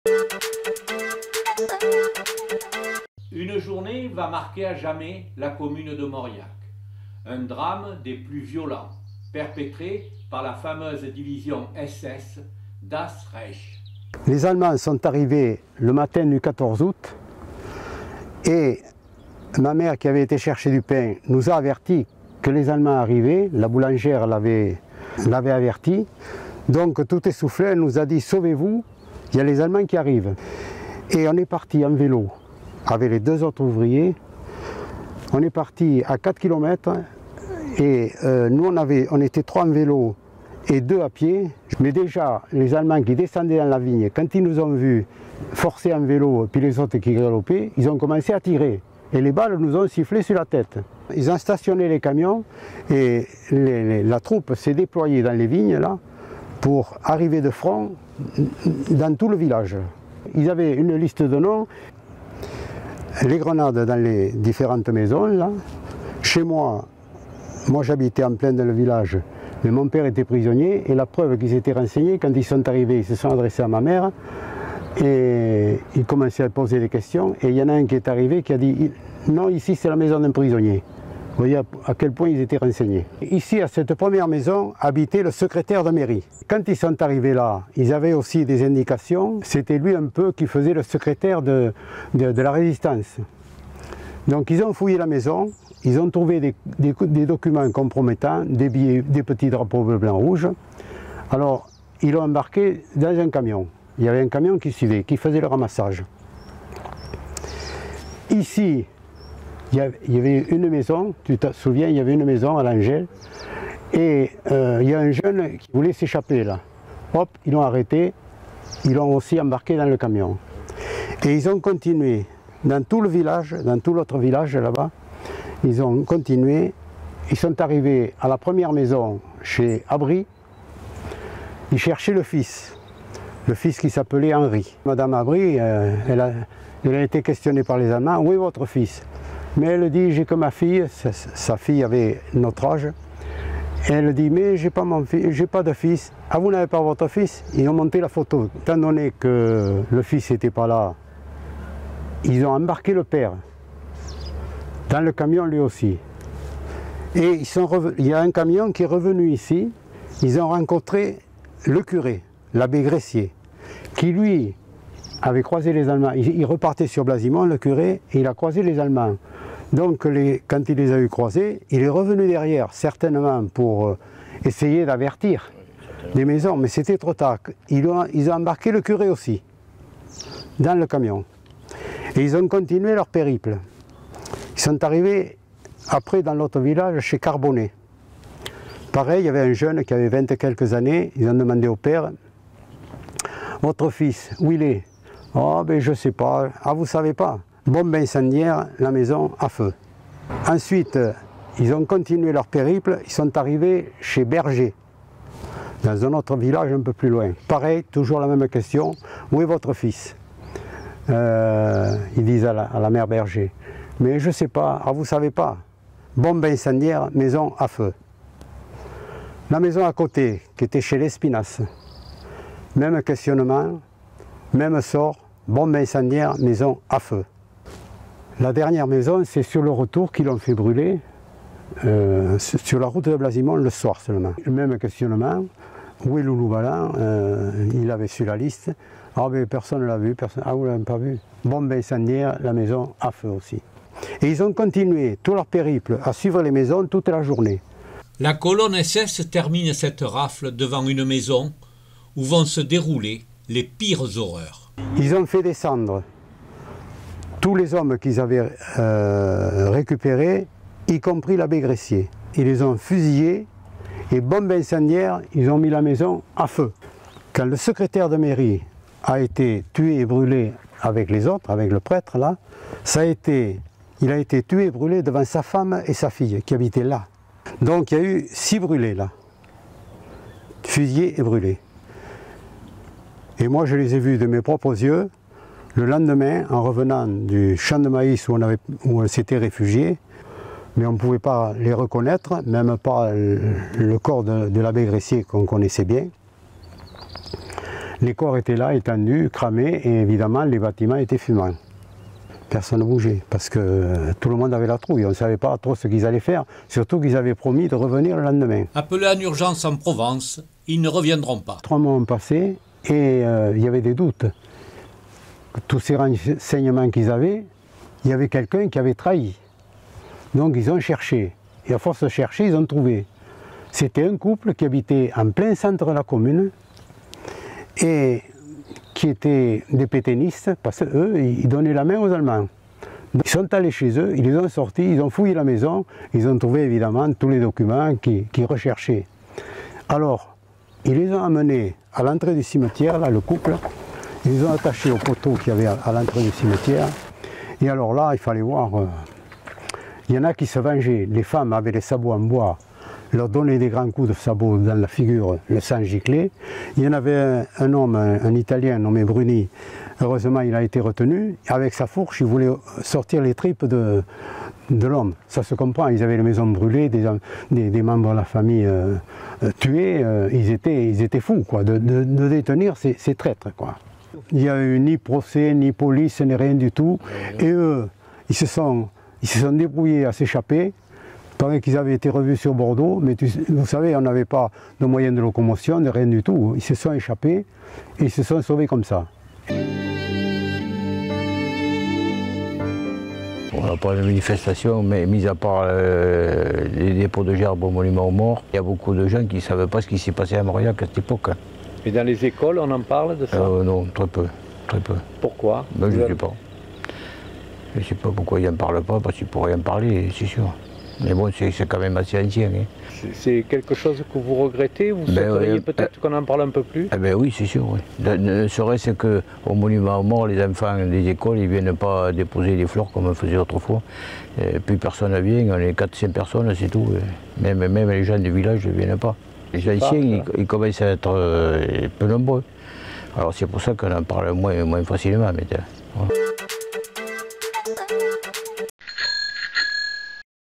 Les massacres de Mauriac
Francis Farges avait 11 onze ans, lorsque la fameuses  division SS Das Reich  passe et s'arrête dans son village. Les SS renseignés par la milice pétainiste va se livrer aux pires exactions, une violence inouïe qui va marquer à jamais la commune de Mauriac . Tortures exécutions incendies un épisode de la guerre raconté par celui  qui à vécu ses moments dramatiques, Francis Farges. Il est  accompagné du Maire  de Mauriac , Jean-Marie Viaud